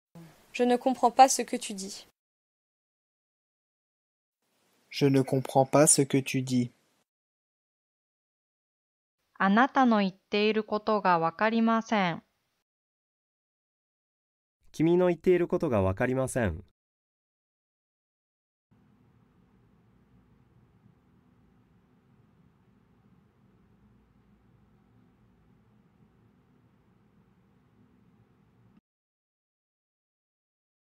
フランス語で"君の言っていることが分かりません。"の発音の仕方
男性コーチ
Je ne comprends pas ce que tu dis.

君の言っていることが分かりません。

あなたの言っていることが分かりません。